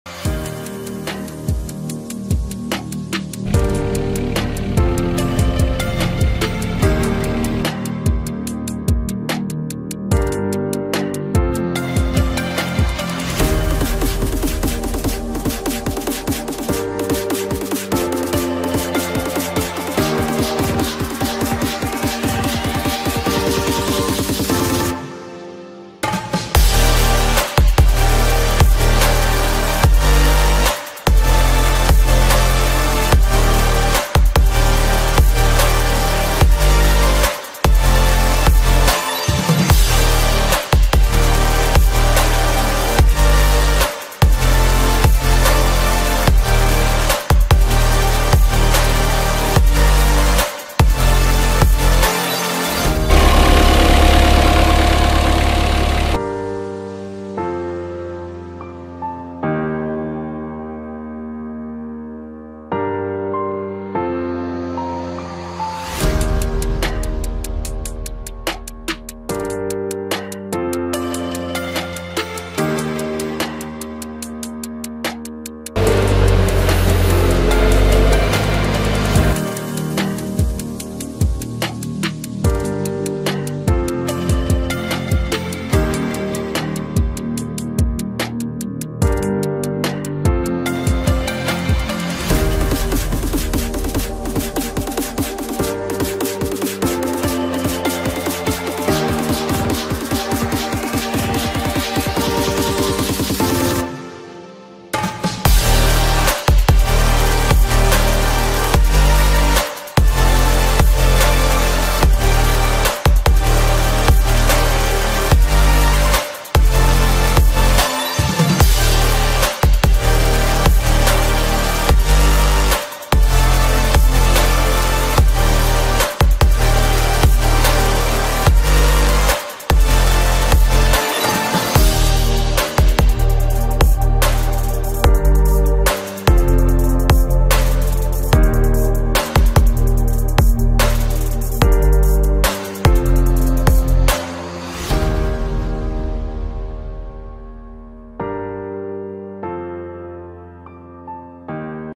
21 Miesięcy ładowarki Eurotrac W12S na gospodarstwie
Witajcie w dzisiejszym filmiku przedstawienie naszej ładowarki podczas jej codziennych obowiązków.
Pozdrawiam